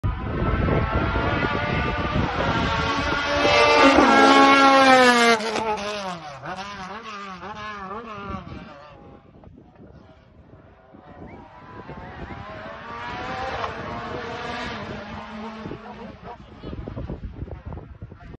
Montée historique ballon d Alsace 2022. Cedric Grosjean .
1 er essai avec cette auto...